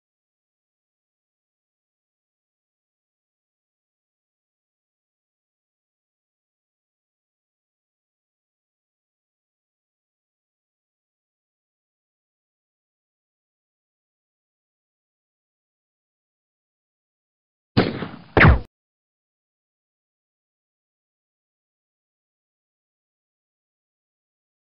bow and arrow awesomeness
awesome gun battle with knives and bow and arrows